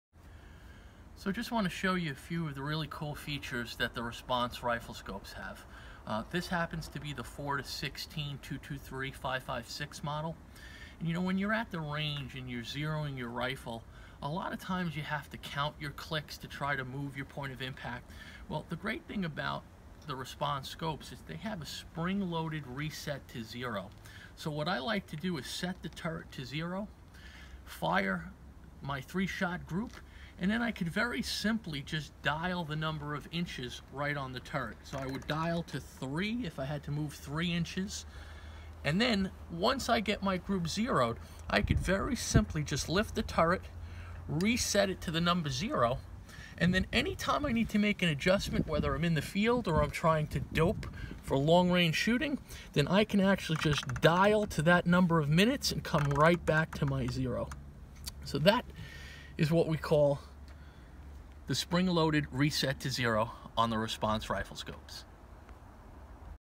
TRACT RESPONSE AR Riflescope Reset to Zero
This video demonstrates the simplicity of resetting the TRACT  RESPONSE AR riflescope turrets to zero